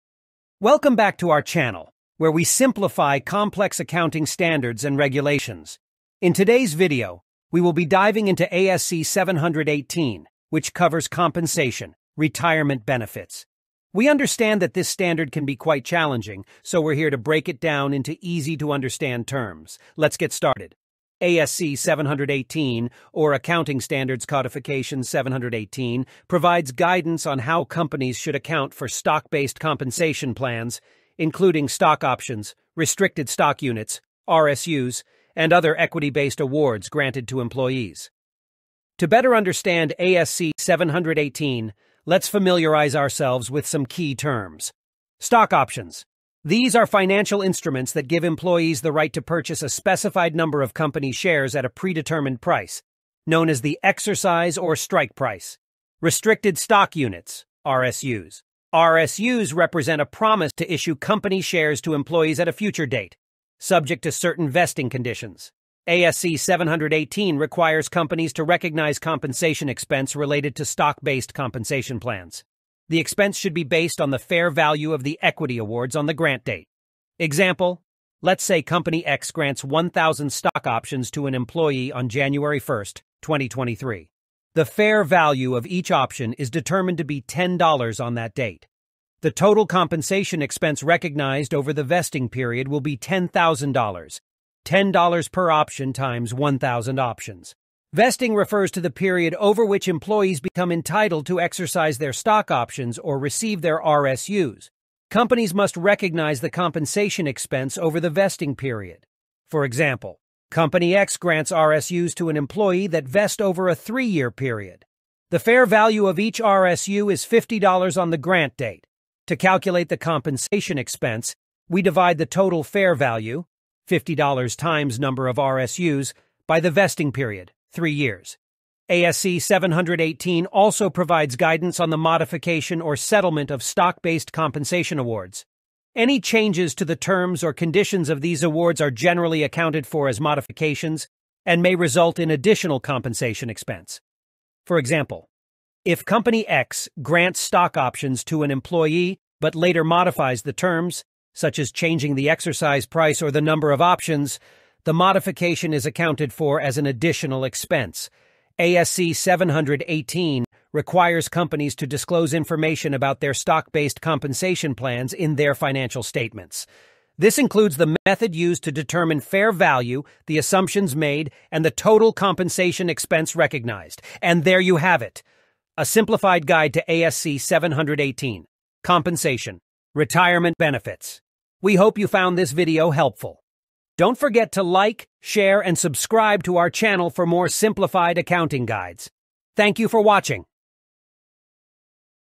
ASC 718 Simplified!!!
In this video, we simplify ASC 718: Compensation - Retirement Benefits, a complex accounting standard that covers stock-based compensation plans. We break down the key concepts and provide real-world examples to help you understand how to account for stock options, restricted stock units (RSUs), and other equity-based awards granted to employees. Whether you're a CPA, an accounting student, or a business owner, this comprehensive guide will make ASC 718 more accessible. Join us as we simplify accounting regulations and make them easier to comprehend.